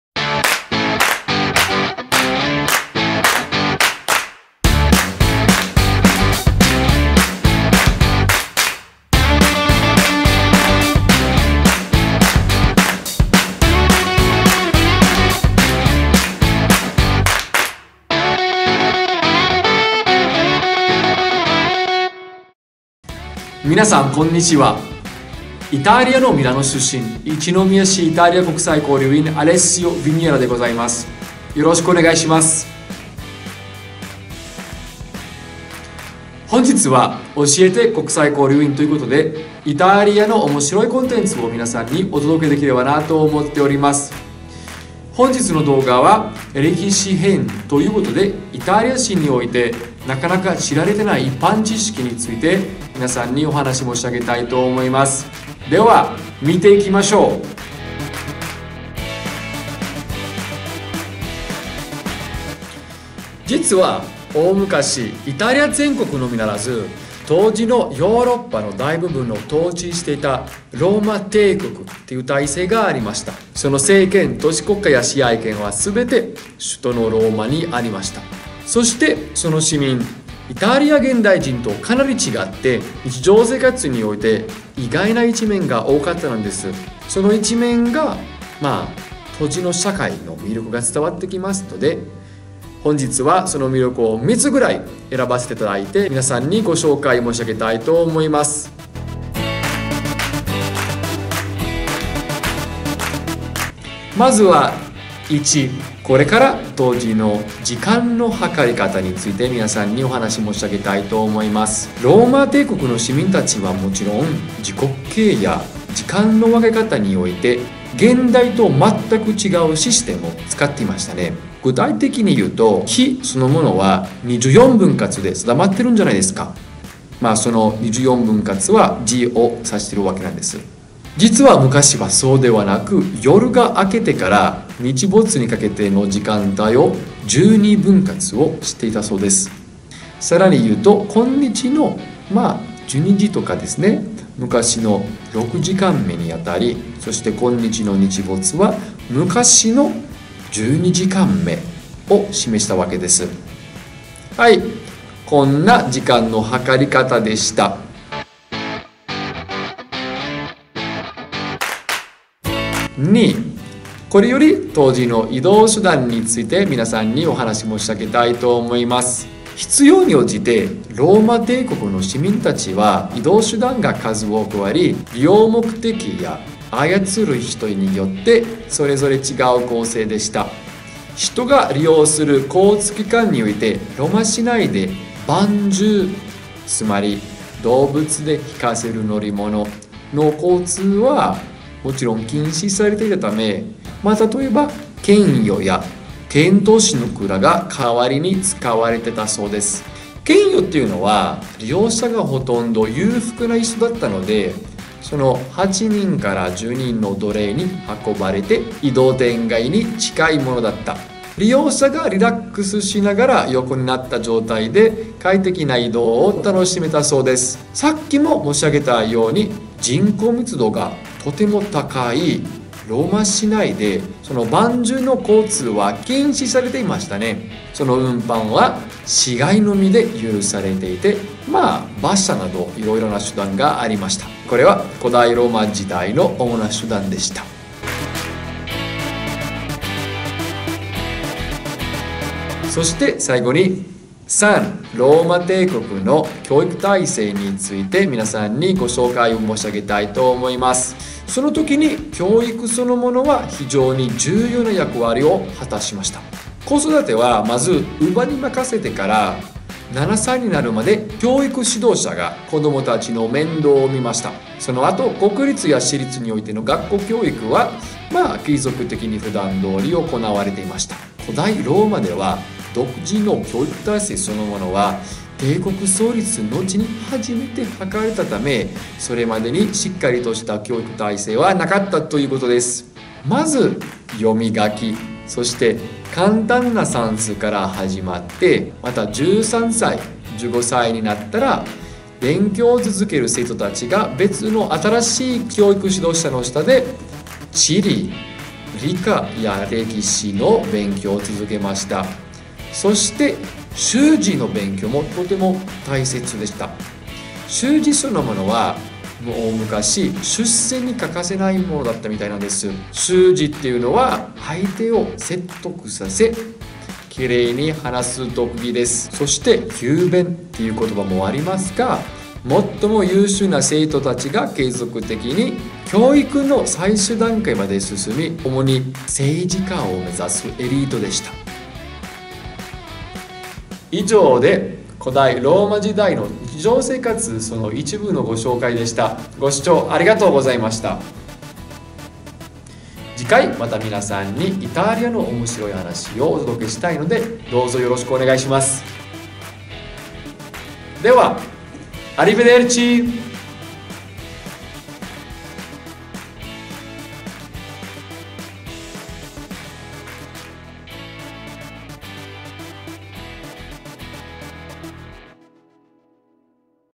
【教えて！国際交流員】イタリア編 第1回 古代ローマ時代の意外な一般知識
一宮市国際交流員のアレッシオ（イタリア出身）が、5分程度の動画で母国の紹介をします！

第一回のテーマは「古代ローマ時代の意外な一般知識」です。